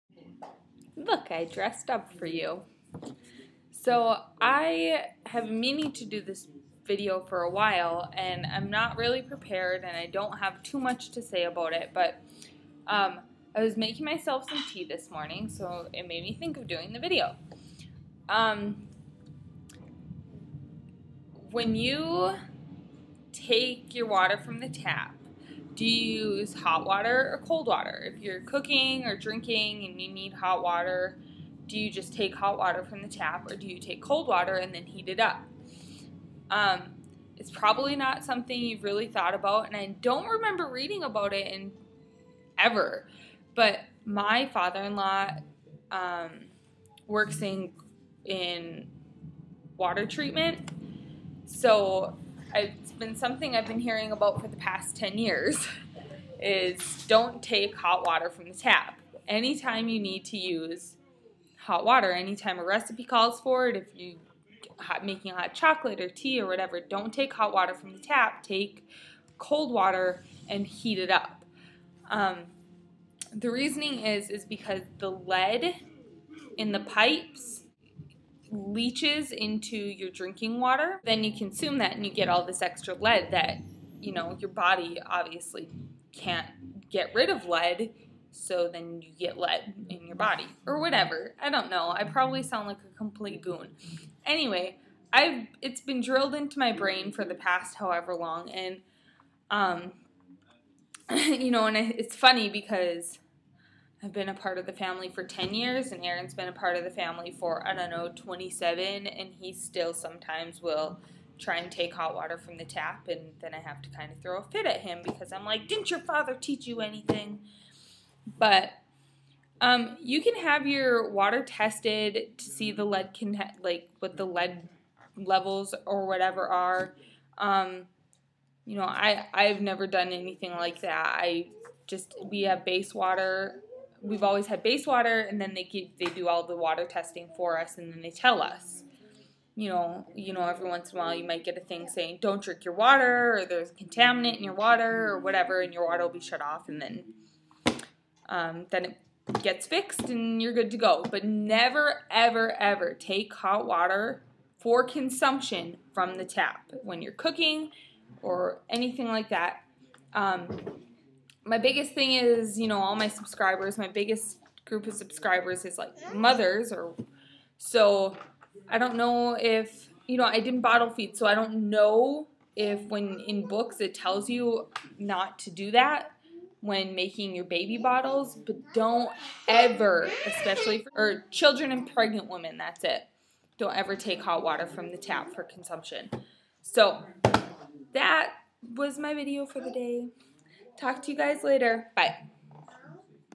Lead in your drinking water
I'm no expert,  but it has been drilled into my head not to take hot/warm water for consumption from the tap.  The link below can give you a lot more detailed information.  Arm yourself with knowledge! :)